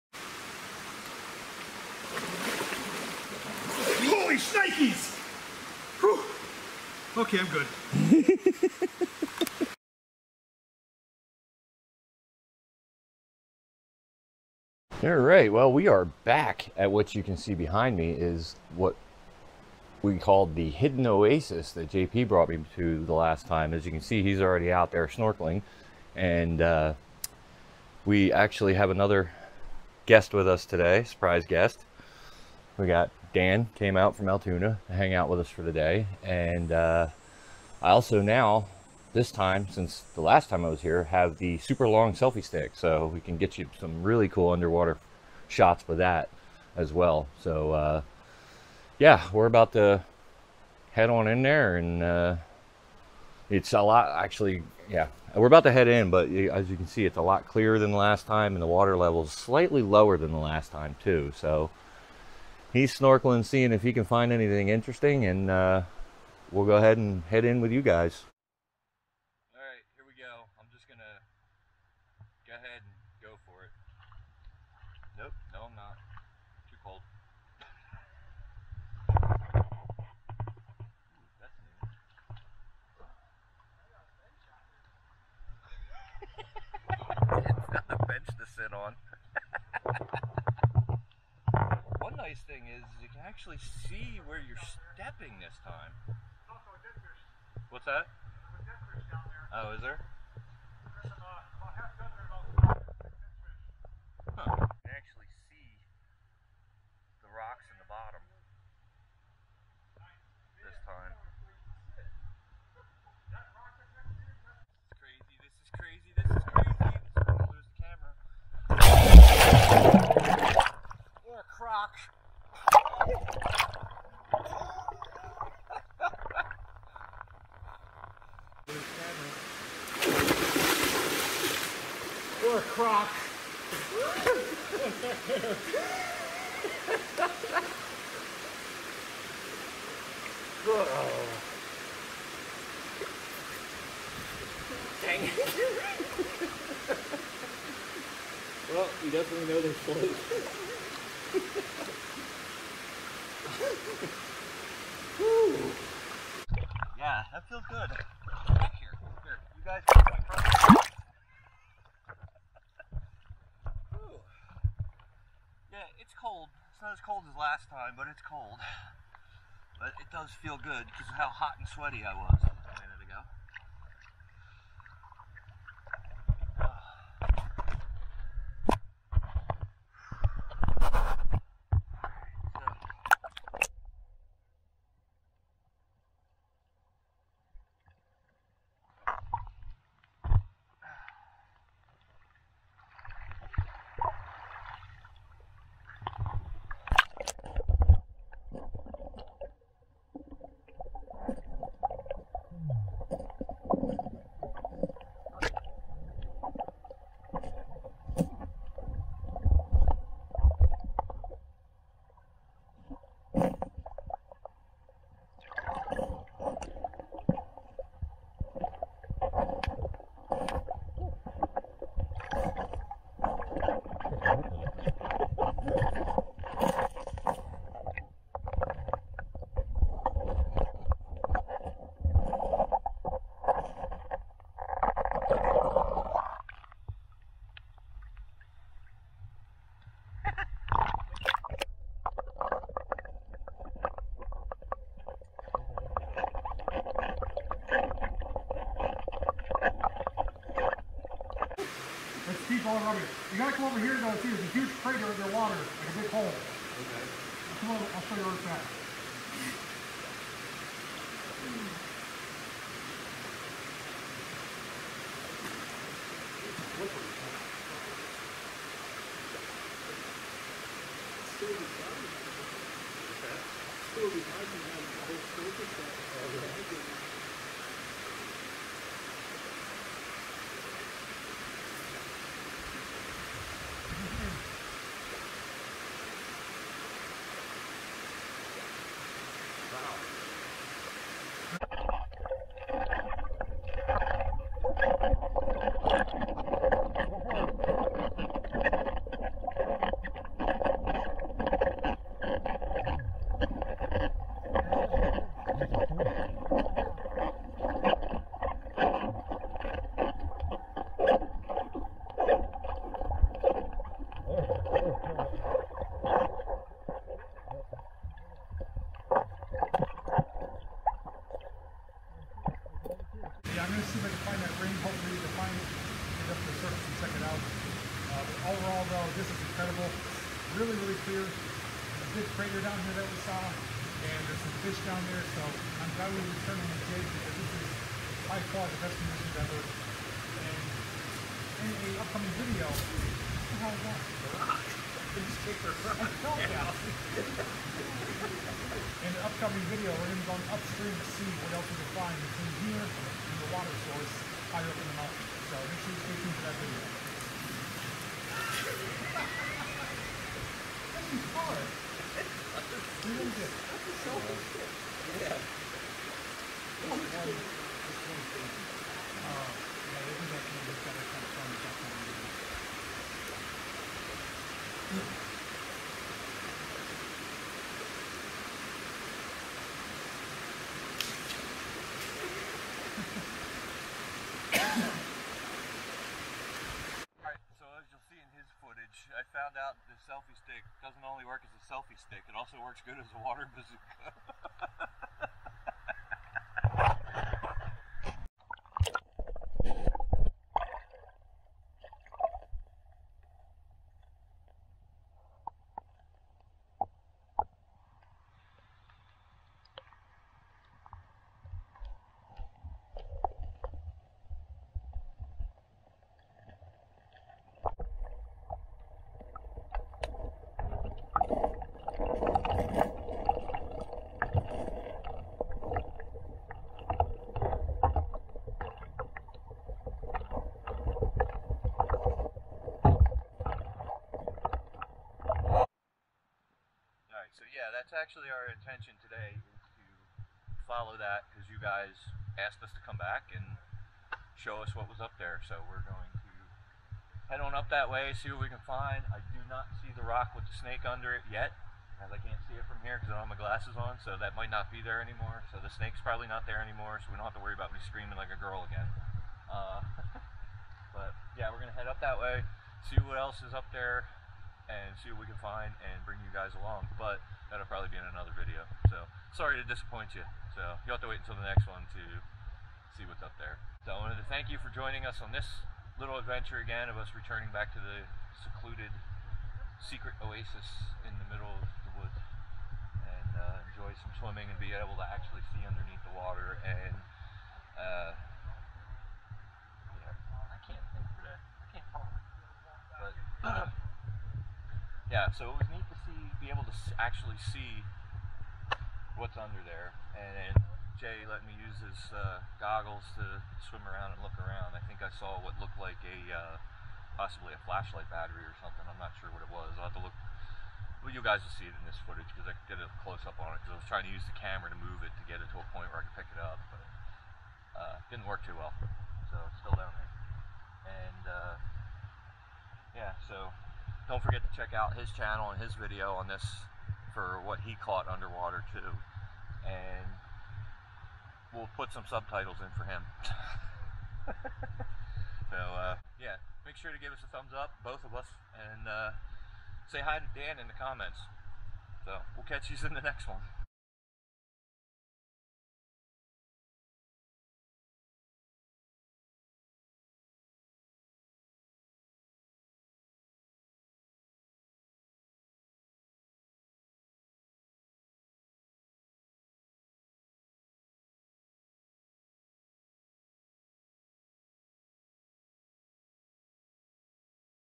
Returning to the hidden oasis with JPVideos and Special Guest
As requested by you the viewers, JP and myself and special guest return to the Hidden Oasis. Here we enjoy the clear water before continuing on up stream to find what is along the way. Join us as we cool off and have some laughs and fun.